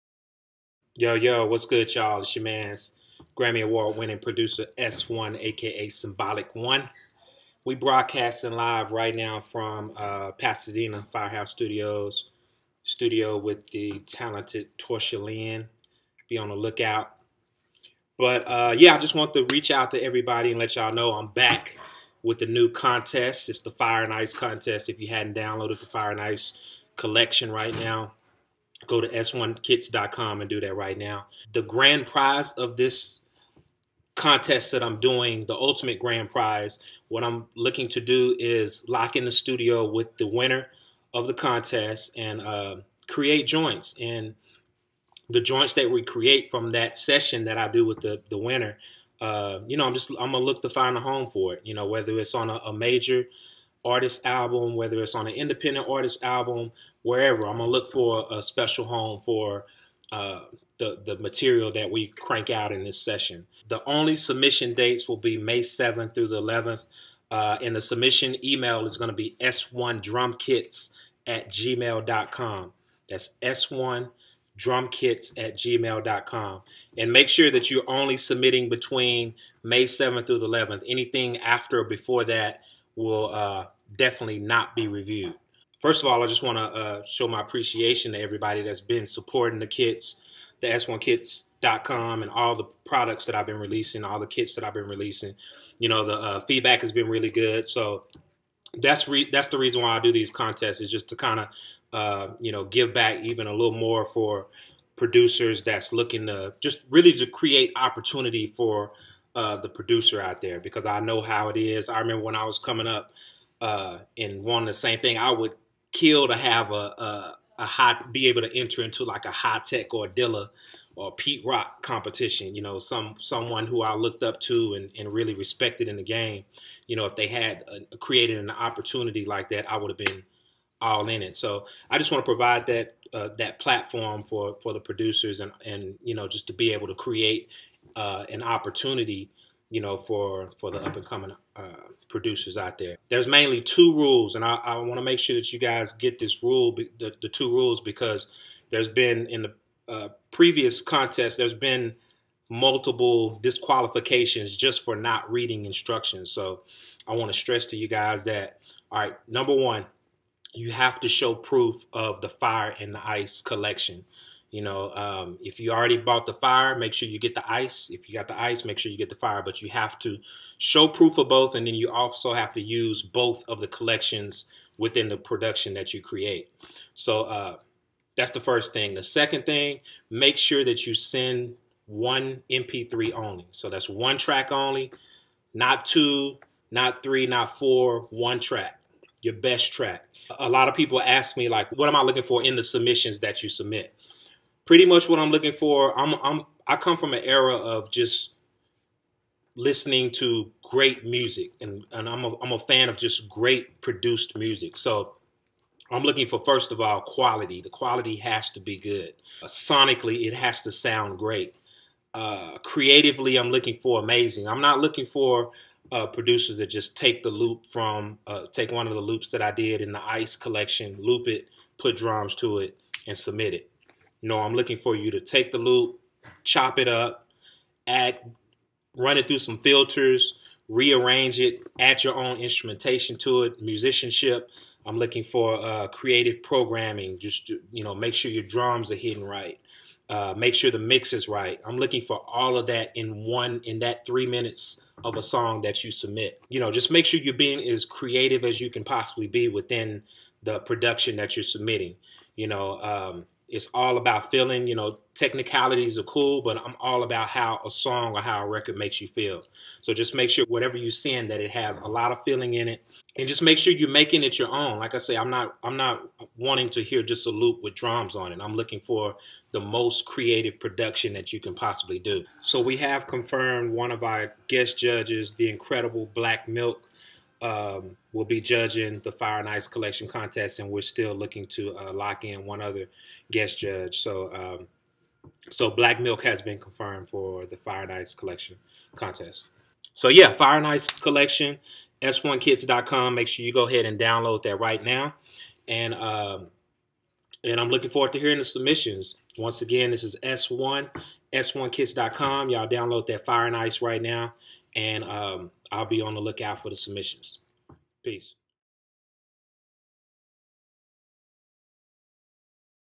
S1Kits.com - Fire and Ice Collection Kit Contest
Producer S1 Speaks about Fire and Ice Collection Kit Contest - Submission Dates ONLY May 7th-11th